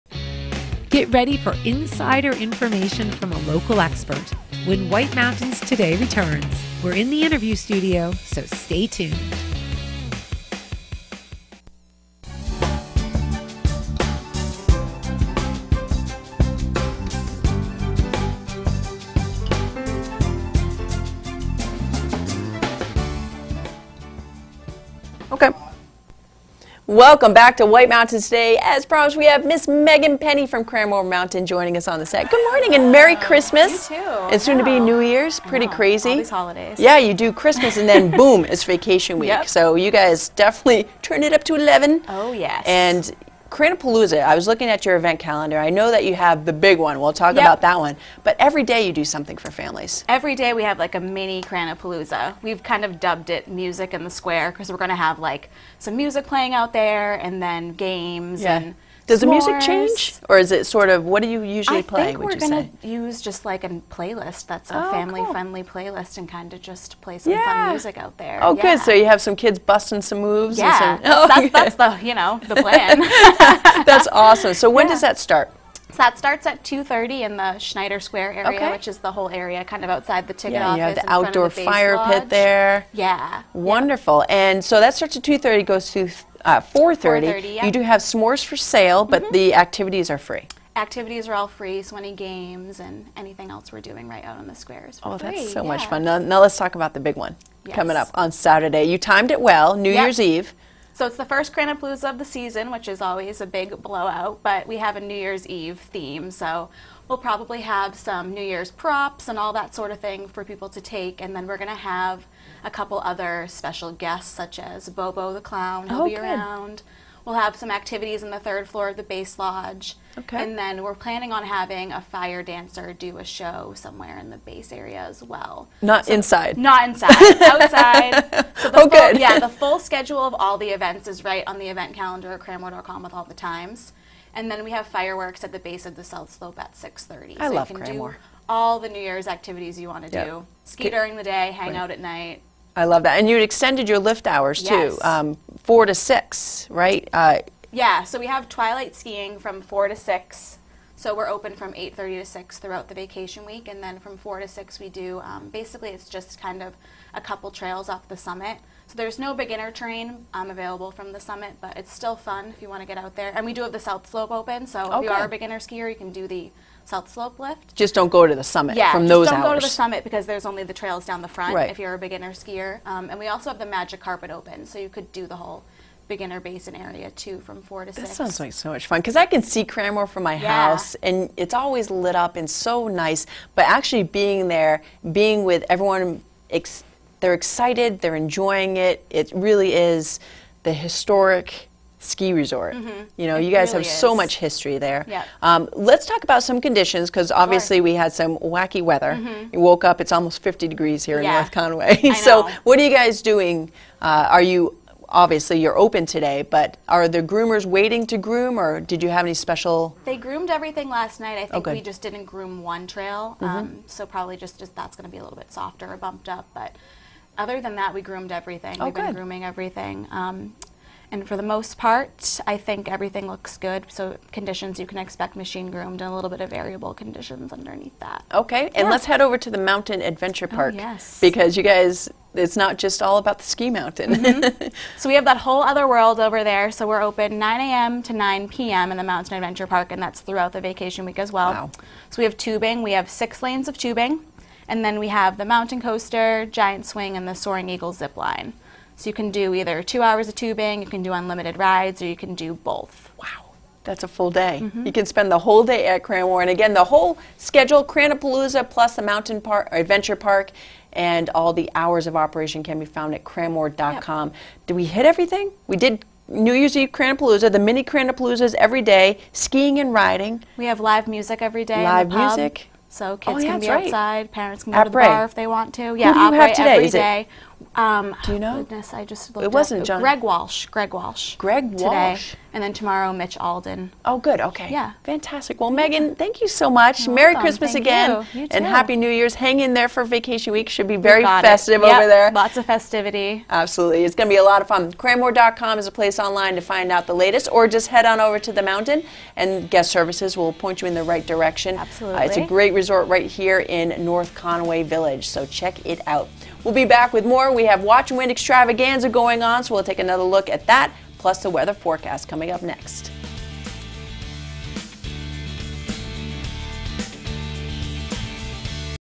Cranmore Cranapalooza, C-More + New Year's Eve Doings
Meghan Penny talks about vacation week. Conditions, events, Adventure Park, Fireworks and more.